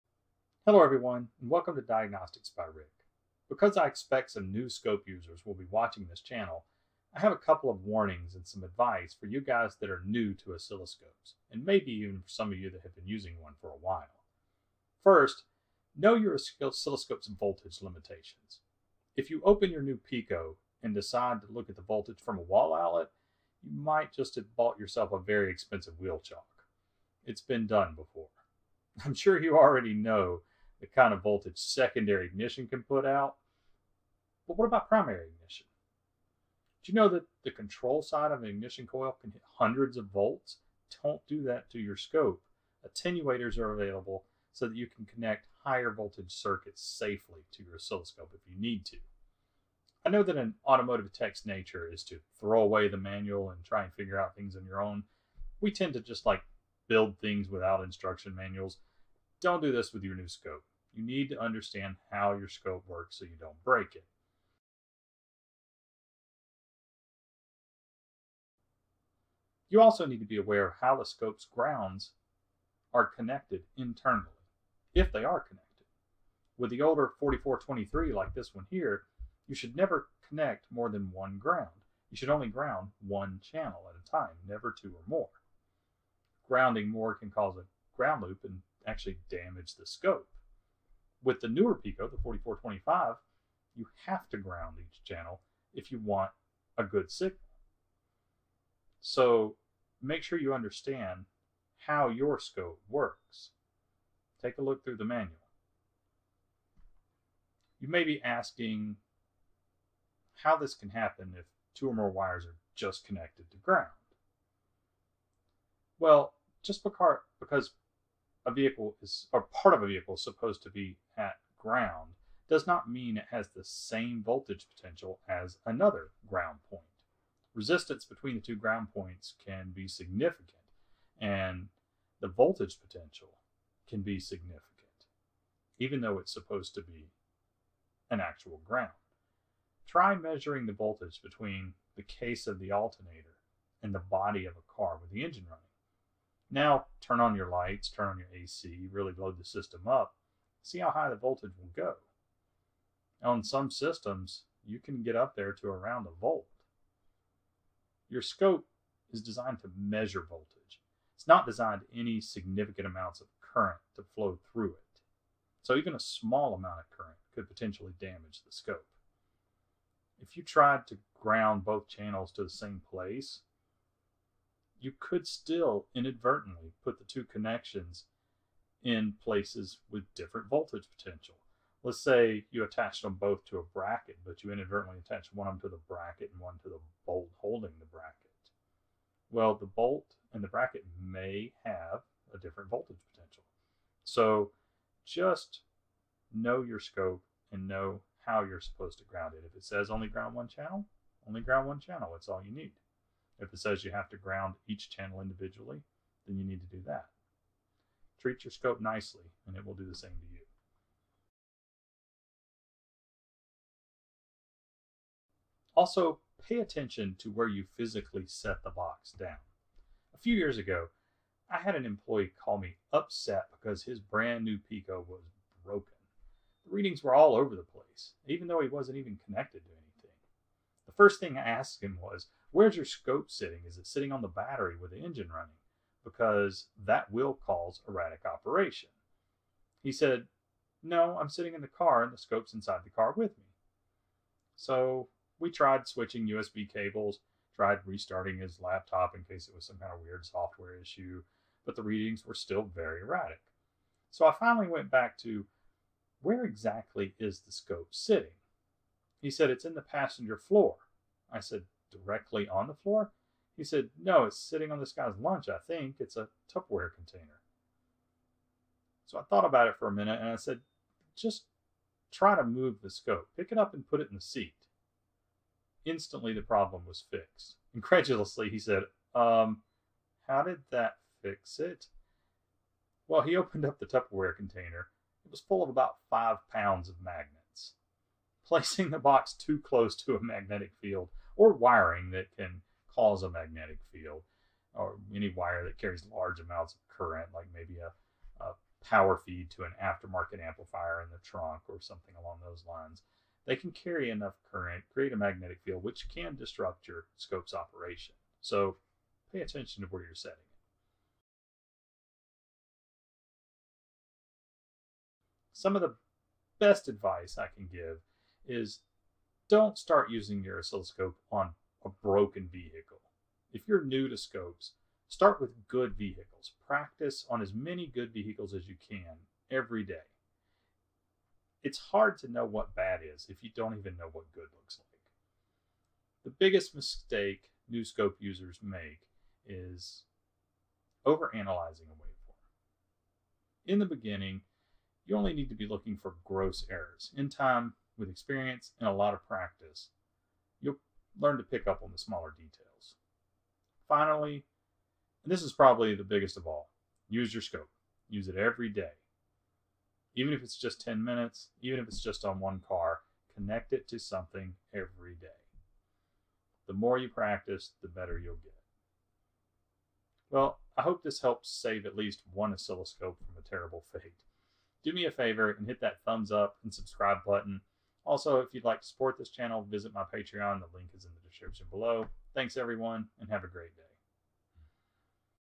Automotive Oscilloscopes Tips and Warnings for New Users
This video contains some tips for new oscilloscope users in the automotive field, and also some warnings to keep you from damaging your new investment.